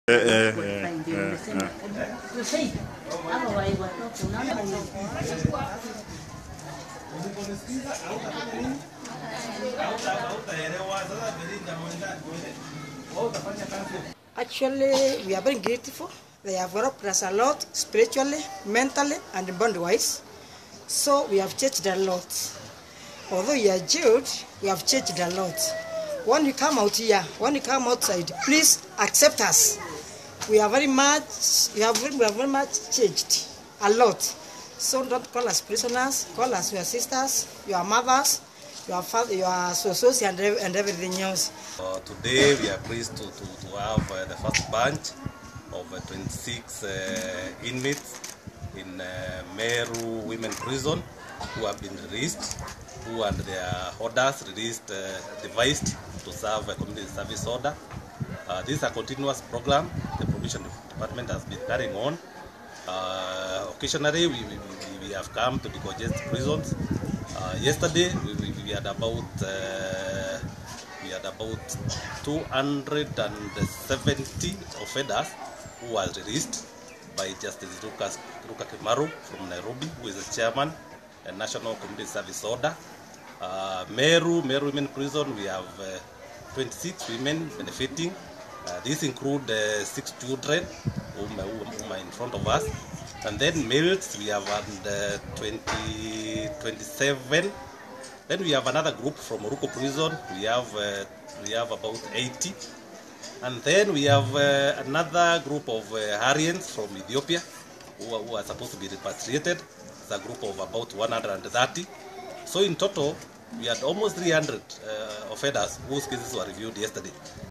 300 inmates released under prison decongestion programme
About 300 inmates have been released from three prisons in Meru County.
Most of those released were those  convicted for committing petty offenses and aliens from Ethiopia who were found guilty of being in the country illegally.
At Kangeta GK prison in Igembe Central 130 Ethiopians handed a one year sentence in August after they failed to pay a fine of Sh 50,000 each will be repatriated back to their country.